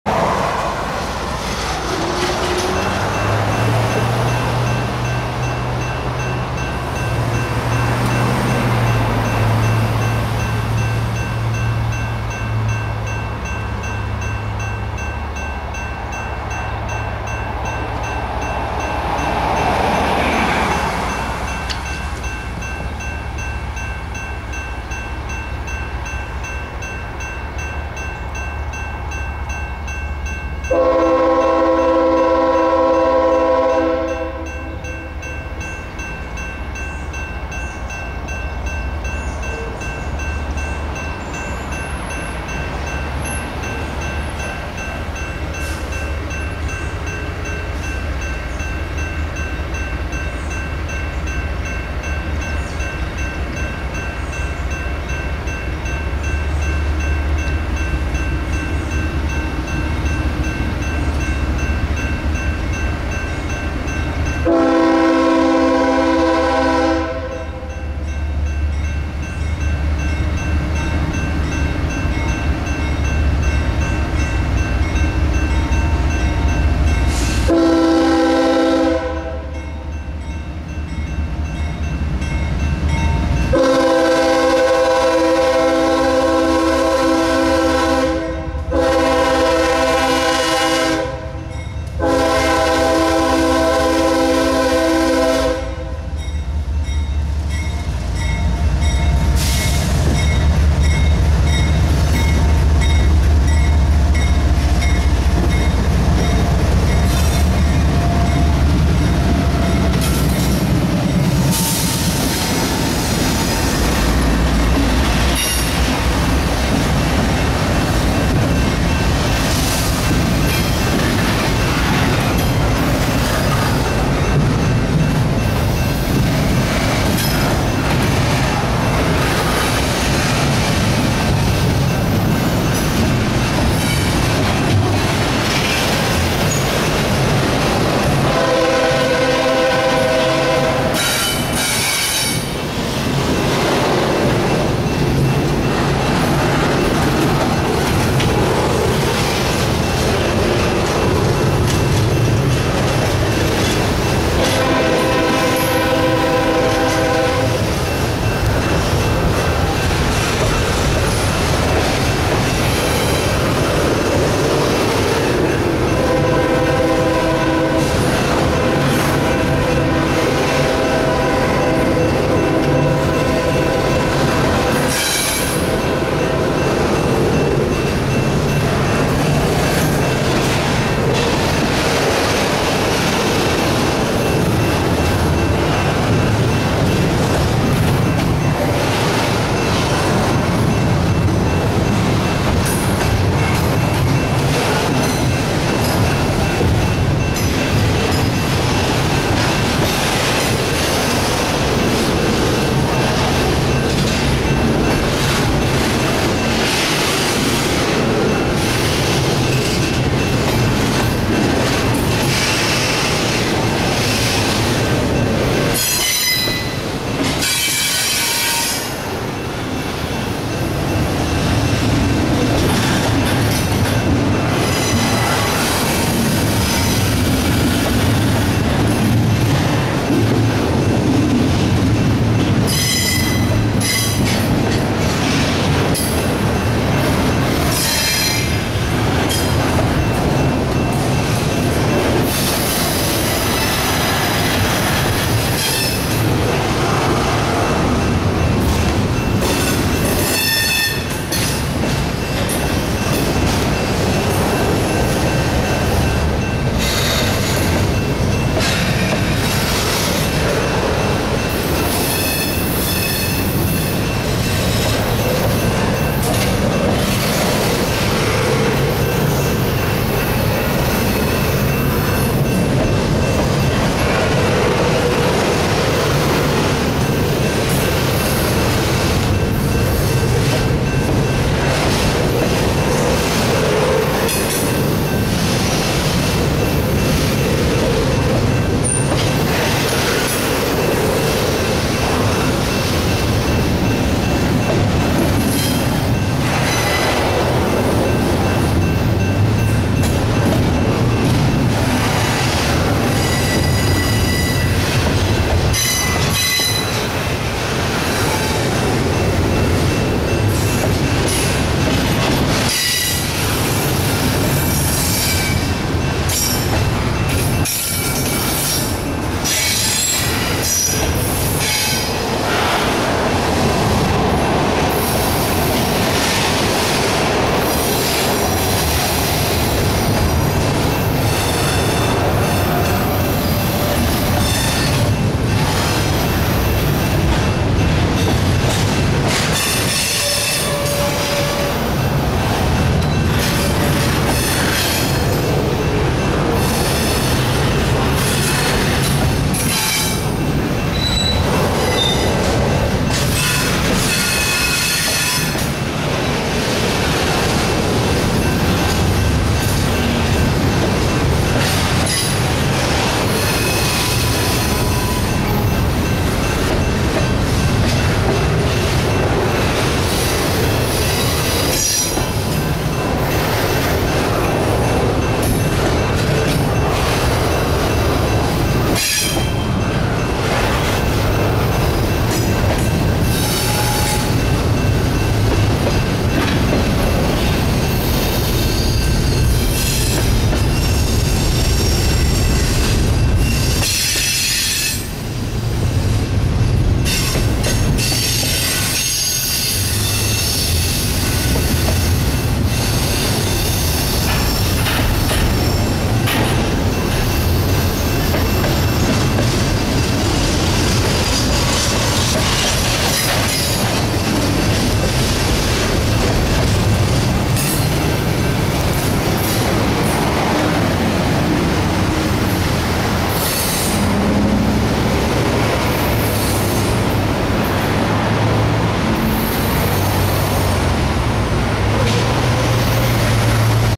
CSX eastbound intermodal train Q194 crossing Dekalb Ave in Decatur,GA
CSX eastbound intermodal train Q194 crossing Dekalb Ave in Decatur,GA just after leaving Hulsey Yard. This train is coming off the old Georgia Road on a branch that connects to the Abbeville Sub. Marta is above the tracks. The tracks basically split the line between the cities of Atlanta and Decatur. Taken around 1:45pm on Thursday, 7/07/11.